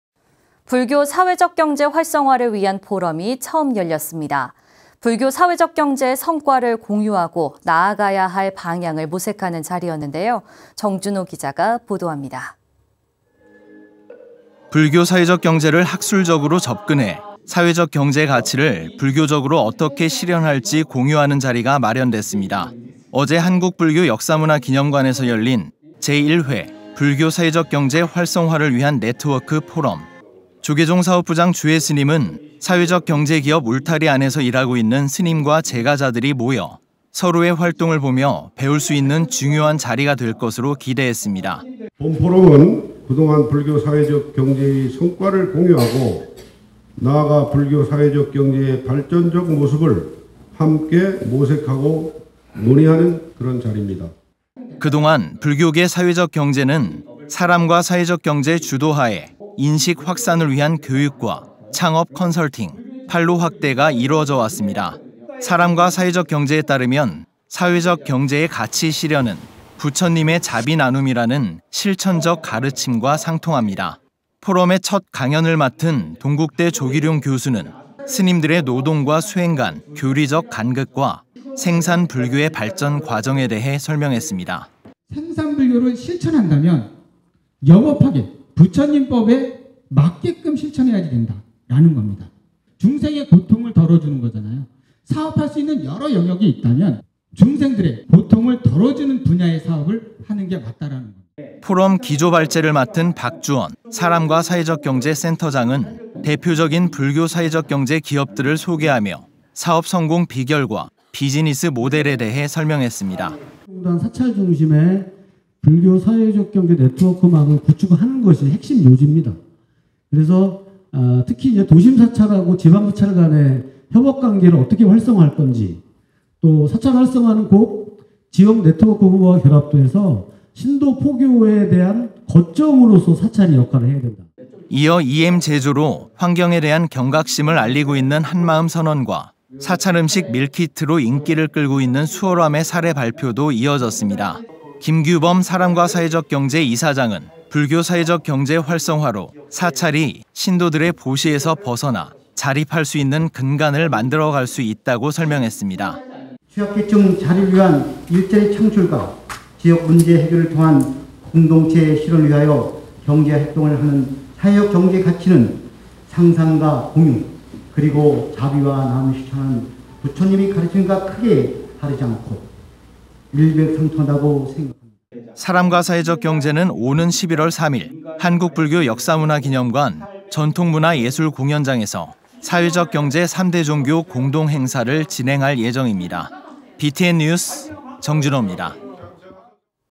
[BTN뉴스] ‘불교사회적경제 활성화’ 논의 시작됐다
〔앵커〕

불교사회적경제 활성화를 위한 포럼이 처음 열렸습니다. 불교사회적경제의 성과를 공유하고 나아가야 할 방향을 모색하는 자리였는데요, 정준호 기자가 보도합니다.
〔리포트〕

불교사회적경제를 학술적으로 접근해 사회적경제 가치를 불교적으로 어떻게 실현할지 공유하는 자리가 마련됐습니다.

어제 한국불교역사문화기념관에서 열린 ‘제1회 불교사회적경제 활성화를 위한 네트워크 포럼’.

조계종 사업부장 주혜스님은 사회적경제기업 울타리 안에서 일하고 있는 스님과 재가자들이 모여 서로의 활동을 보며 배울 수 있는 중요한 자리가 될 것으로 기대했습니다.

주혜스님 / 조계종 사업부장
(본 포럼은 그동안 불교사회적경제의 성과를 공유하고 나아가 불교사회적경제의 발전적 모습을 함께 모색하고 논의하는 자리입니다.)

그동안 불교계 사회적경제는 사람과사회적경제 주도하에 인식 확산을 위한 교육과 창업 컨설팅, 판로확대가 이뤄져 왔습니다.

사람과사회적경제에 따르면 사회적경제의 가치실현은 부처님의 ‘자비 나눔’이라는 실천적 가르침과 상통합니다.

포럼의 첫 강연을 맡은 동국대 조기룡 교수는 스님들의 노동과 수행 간 교리적 간극과 생산 불교의 발전과정에 대해 설명했습니다.

조기룡 / 동국대학교 교수
(생산 불교를 실천한다면 여법하게 부처님 법에 맞게끔 실천해야 된다는 겁니다. 중생의 고통을 덜어주는 거잖아요. 사업할 수 있는 여러 영역이 있다면 중생들의 고통을 덜어주는 분야의 사업을 하는 게 맞다는 겁니다.)

포럼 기조 발제를 맡은 박주언 사람과사회적경제 센터장은 대표적인 불교 사회적경제 기업들을 소개하며 사업 성공 비결과 비즈니스 모델에 대해 설명했습니다.

박주언 / 사람과사회적경제 센터장
(사찰 중심의 불교사회적경제 네트워크망을 구축하는 것이 핵심 요지입니다. 특히 도심사찰과 지방사찰간의 협업 관계를 어떻게 활성화 할 건지, 사찰활성화는 곧 지역네트워크와 결합돼서 신도포교에 대한 거점으로서 사찰이 역할을 해야 합니다. )

이어 EM제조로 환경에 대한 경각심을 알리고 있는 한마음선원과 사찰음식 밀키트로 인기를 끌고 있는 수월암의 사례발표도 이어졌습니다.

김규범 사람과사회적경제 이사장은 불교사회적경제 활성화로 사찰이 신도들의 보시에서 벗어나 자립할 수 있는 근간을 만들어 갈 수 있다고 설명했습니다.

김규범 / 사람과사회적경제 이사장
(취약계층 자립을 위한 일자리 창출과 지역문제 해결을 통한 공동체 실현을 위해서 경제활동을 하는 사회적경제의 가치는 상생과 공유, 그리고 자비 나눔을 실천하는 부처님의 가르침과 크게 다르지 않고 일맥상통한다고 생각합니다. )

사람과사회적경제는 오는 11월 3일 한국불교역사문화기념관 전통문화예술공연장에서 사회적경제 3대 종교 공동행사를 진행할 예정입니다.

BTN 뉴스 정준호입니다.